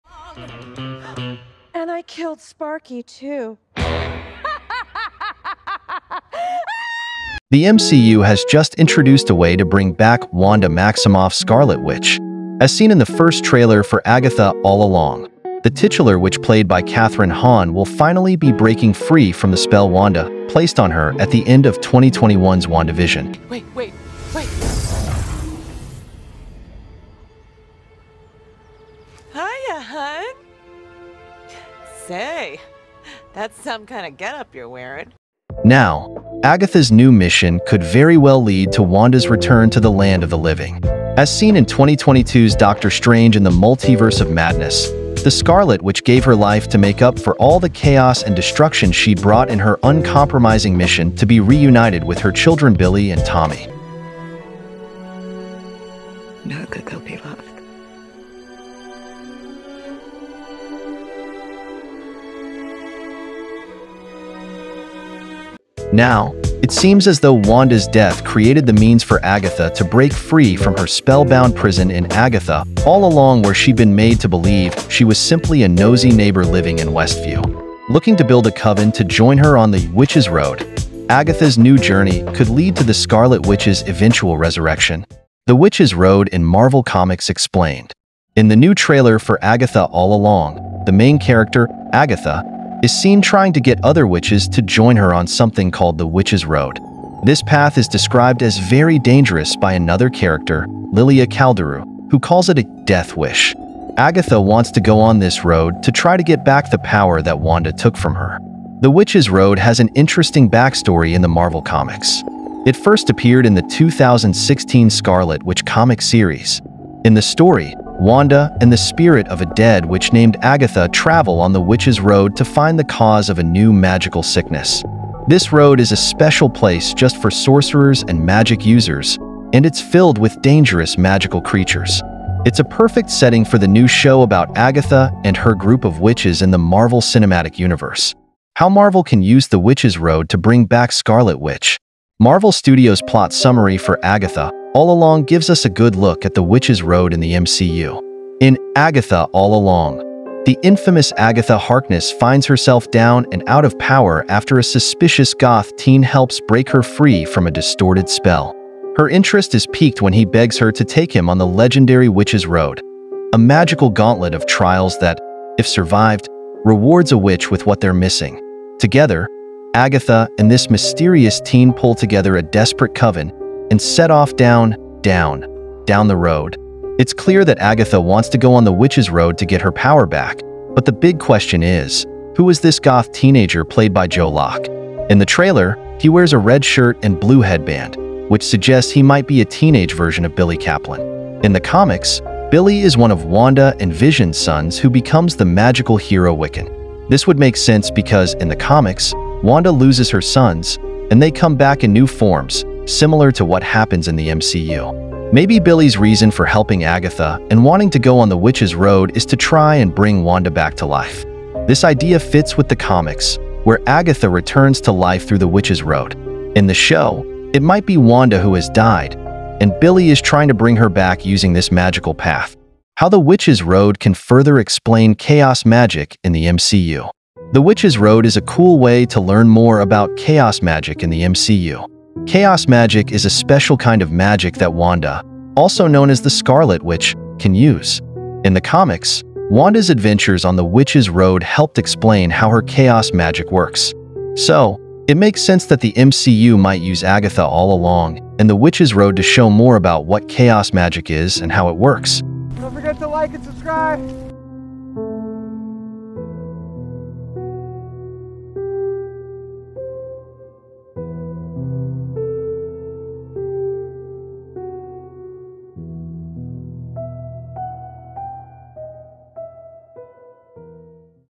How Agatha All Along Brings Wanda Back to life
Agatha All Along is an upcoming American television miniseries created by Jac Schaeffer for the streaming service Disney+, based on the Marvel Comics character Agatha Harkness.